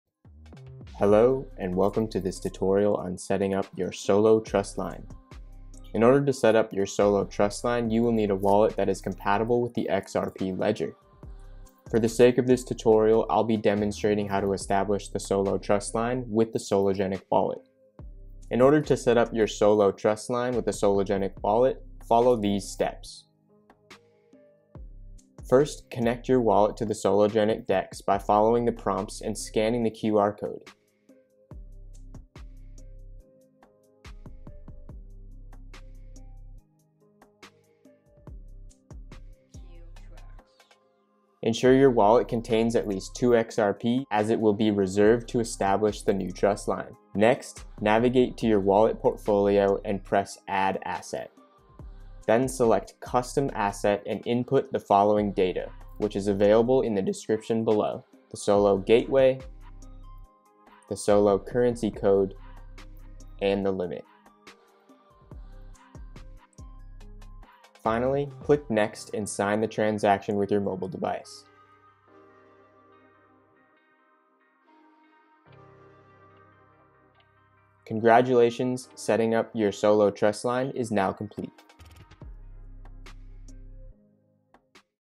Tutorial: How To Set The SOLO Trustline - Sologenic Wallet
In order to set up your SOLO trust line with the Sologenic wallet, follow these steps:

Step 1:

Connect your wallet to the Sologenic Dex by following the prompts and scanning the QR code. Ensure your wallet contains at least 2 XRP as it will be reserved to establish the new trustline.

Step 2:

Navigate to your wallet portfolio and press Add Asset.

Step 3:

Select custom asset and Input the following data, which is available in the description below:

SOLO Gateway (Issuer):
rsoLo2S1kiGeCcn6hCUXVrCpGMWLrRrLZz

SOLO Currency Code:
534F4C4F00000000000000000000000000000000

Limit:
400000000

Step 4:

Click next and sign the transaction with your mobile device. Setting up your SOLO trust line is now complete.

Found this video helpful?
XRP: rKcSLRowEqTCWTbEhHdgTY44nMgdQQdokM